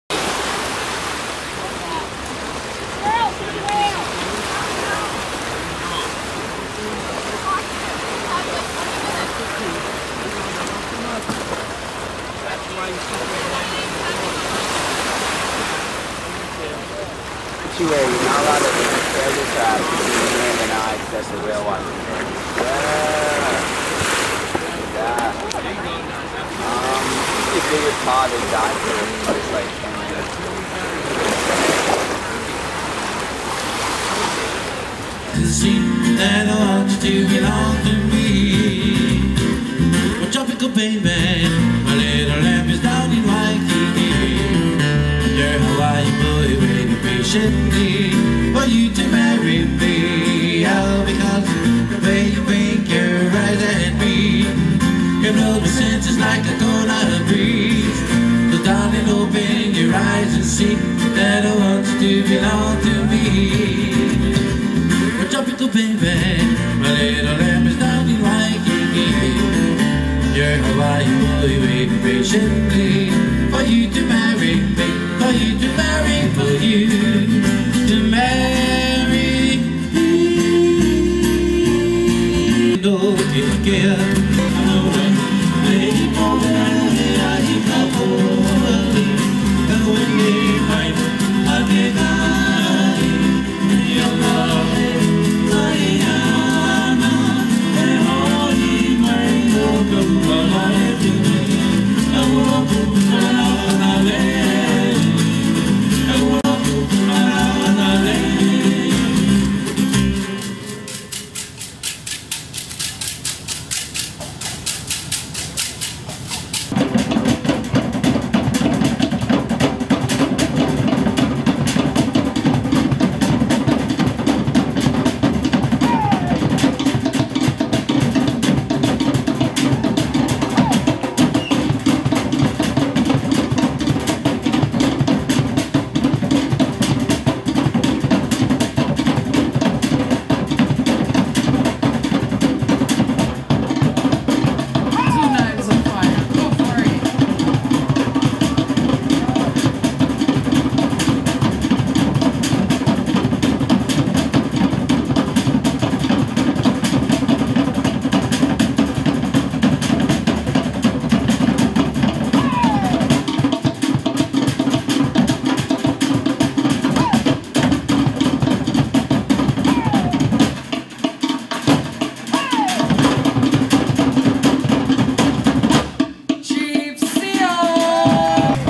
NATALIE_HAWAII_LUA_whales
This trip to MAUI was in March and we saw humpback whales, went surfing, and attended a real lua. travel and places catgegory